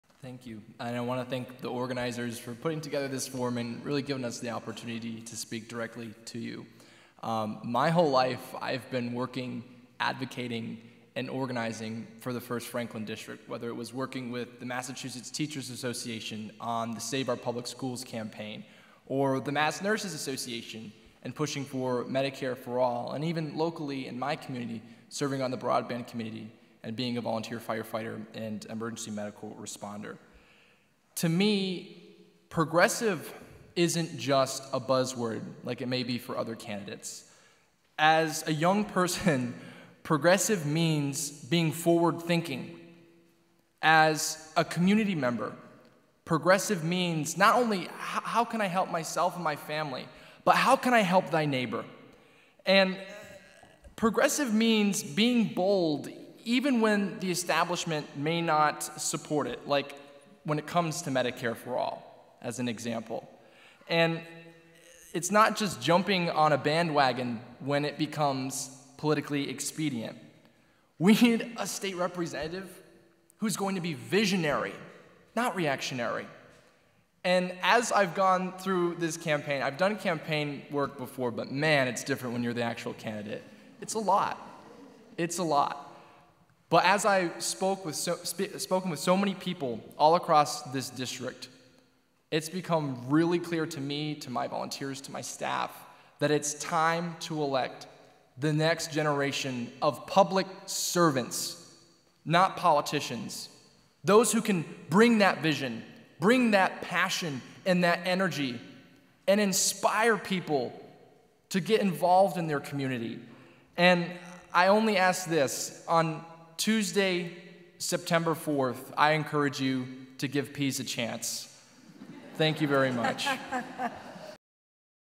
Casey Pease's Closing Remarks - 1st Franklin District (MA) Candidate's Forum - 8/7/18
State Rep. candidate Casey Pease's closing remarks from the 1st Franklin District Candidate's Forum put on by the Franklin County League of Women Voters, the Greenfield Recorder, & FCAT. Recorded on August 7th, 2018 at The Hess Center at Deerfield Academy.